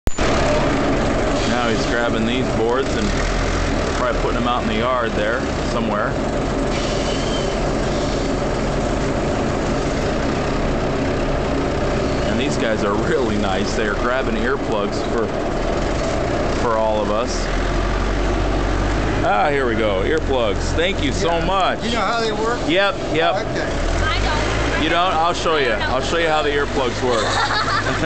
Stacking the lumber
Mike taking it off the dock and stacking it out in the yard